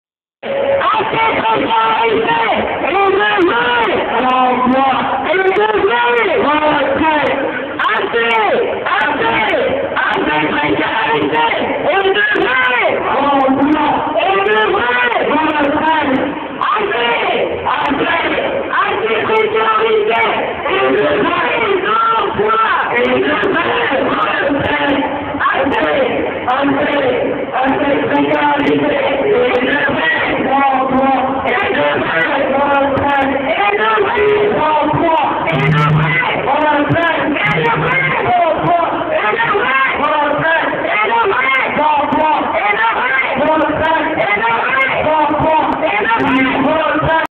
Lycéens en marche
06/11/2010 . Manifestation contre la réforme des retraites. Les lycéens boulevard Voltaire dans le second cortège de la manifestation parisienne.
enregistrement: yann beuvais